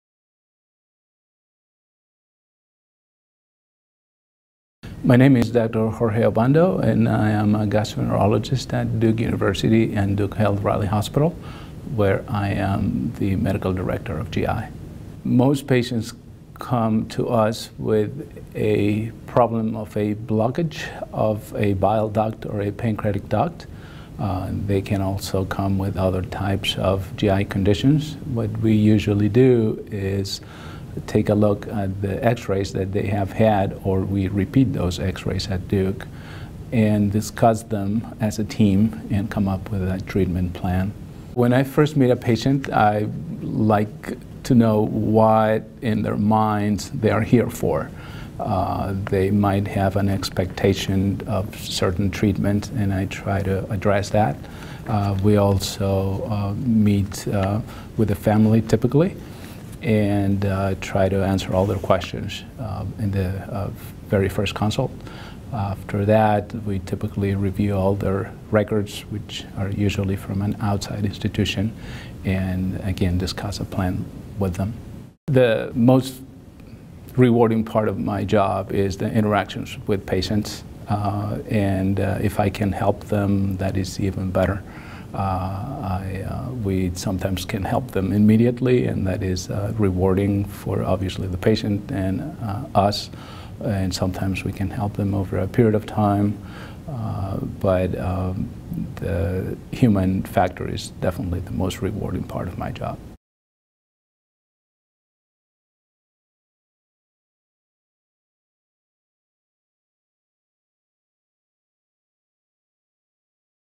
Jorge V. Obando, MD
Gastroenterologist Jorge V. Obando, MD practices at Duke Gastroenterology of Raleigh.

I am a gastroenterologist and the medical director of the gastrointestinal (GI) program at Duke Raleigh Hospital. I enjoy being at Duke, because our team is one of the most experienced in the Southeastern United States. Most of the patients I see have diseases of the biliary or pancreatic systems including blockages. Some of these can be malignant. When I first meet a patient, I like to know their expectations for treatment. I also like to meet the patient’s loved ones and answer any questions they may have at the very first visit. After we talk, I review the patient’s x-rays and records. The next step is to discuss the results with my colleagues as a team, so we can come up with a unique treatment plan that is personalized for each patient. The most rewarding part of my job is the interaction with patients. If I can provide immediate help, I’m even happier.